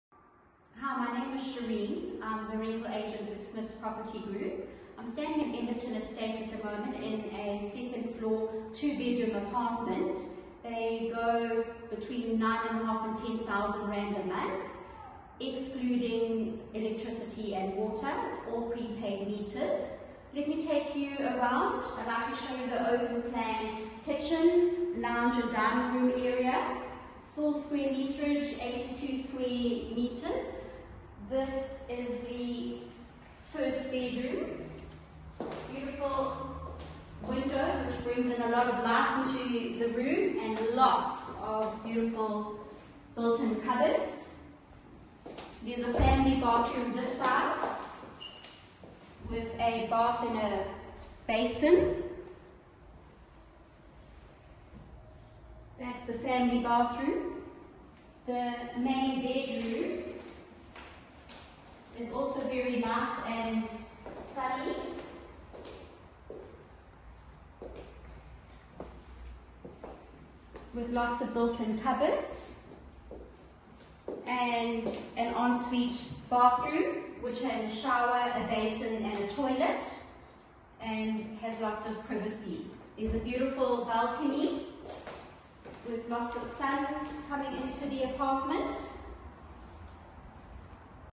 2 Bedroom, 2 Bathroom Apartment in Emberton Estate
This lovely 2-bedroom apartment situated in the popular lifestyle estate of Emberton in Hillcrest is the perfect manageable home for a couple, working professional or for those looking for a lock up and go estate. The modern fittings and bright open plan design make it a warm and cosy place to live.
Make use of the common garden for those evening strolls and the lap pool for those hot summer days. The estate has a great lifestyle attitude with a restaurant/bar; and if tennis, swimming or jogging are part of your lifestyle then Emberton is the estate for you.
Viewings by appointment. Credit checks apply. All units have prepaid electricity, and water is billed separately.